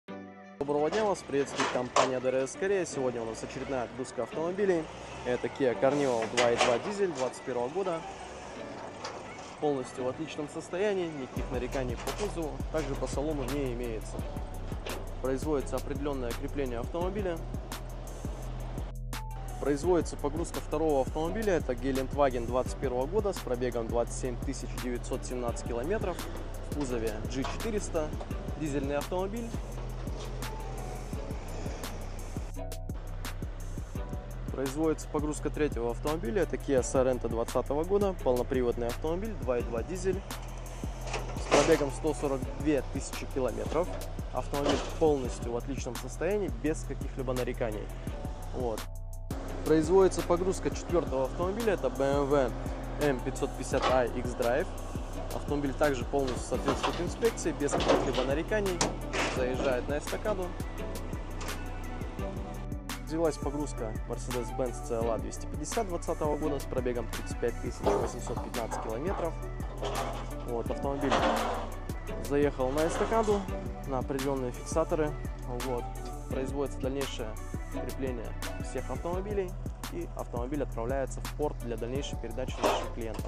Прогрузка автомобилей из Кореи 31.07.2024
На связи DRS Auto Корея 🚀

И наш отчет по отправкам автомобилей из Кореи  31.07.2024

🤝На этой неделе загрузили 5 автомобилей на автовоз и  отправили в порт  в город Пусан для дальнейшей транспортировки судном в страну назначения⤵️

1. Kia Carnival-2021 год
2. Mercedes-Benz Geländewagen G400-2021 год
3. Kia Sorento -2020 год
4. BMW 550i xDrive-2021 год
5. Mercedes-Benz CLA 250-2020 год